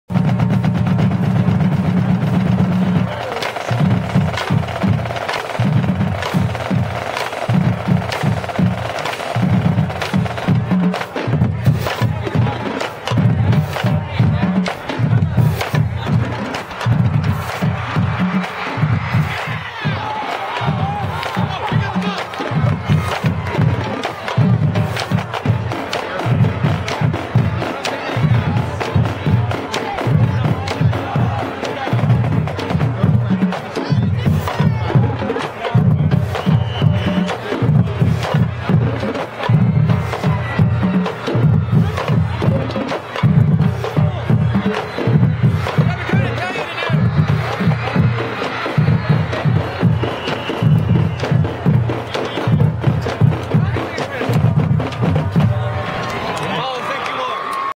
The John Jay Mustang Band performs at the Jay vs Warren football game on Saturday, September 22, 2012.